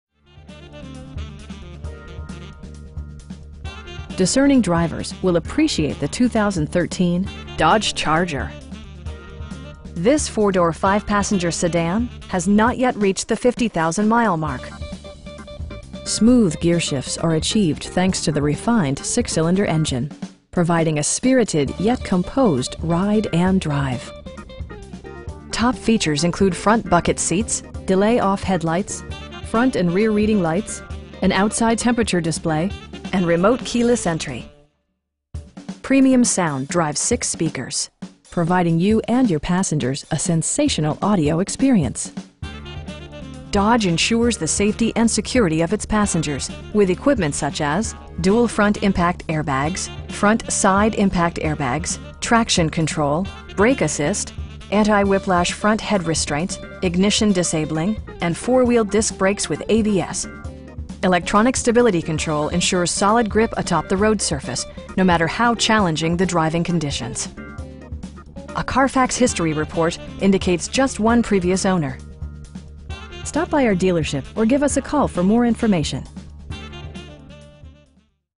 2013 Dodge Charger SE in Montgomery, AL 36117
Brewbaker Preowned
300 Eastern Blvd.

Discerning drivers will appreciate the 2013 Dodge Charger. This 4 door, 5 passenger sedan has not yet reached the 50,000 mile mark! Smooth gearshifts are achieved thanks to the refined 6 cylinder engine, providing a spirited, yet composed ride and drive. Top features include front bucket seats, delay-off headlights, front and rear reading lights, an outside temperature display, tilt and telescoping steering wheel, power door mirrors, power windows, and remote keyless entry. Premium sound drives 6 speakers, providing you and your passengers a sensational audio experience. Dodge ensures the safety and security of its passengers with equipment such as: dual front impact airbags, front SIDE impact airbags, traction control, brake assist, anti-whiplash front head restraints, ignition disabling, and 4 wheel disc brakes with ABS. Electronic stability control ensures solid grip atop the road surface, no matter how challenging the driving conditions. A Carfax history report indicates just one previous owner. We have the vehicle you've been searching for at a price you can afford. Stop by our dealership or give us a call for more information.